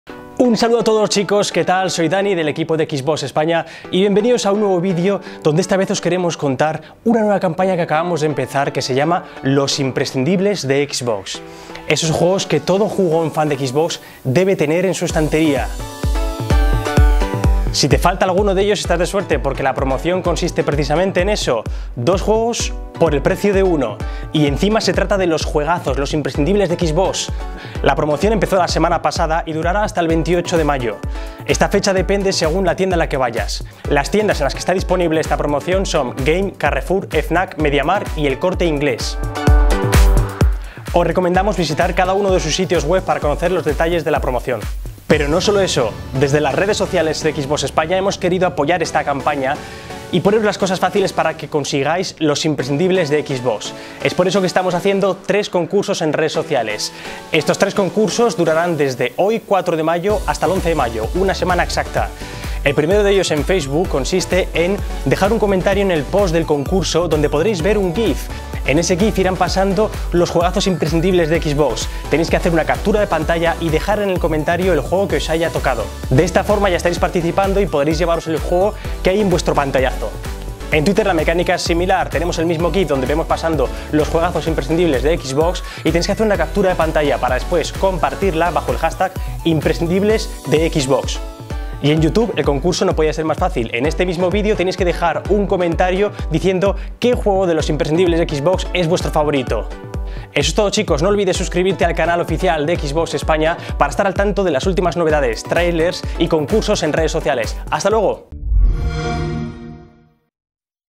Los Imprescindibles de Xbox | Concursos y Promociones
Presentamos "Los Imprescindibles de Xbox", juegazos que todo fan de Xbox debe tener en su estantería. Oferta disponible en GAME, Media Markt, FNAC, Carrefour y El Corte Inglés. ¡Juegos 2x1! Visita la web de cada tienda para más información.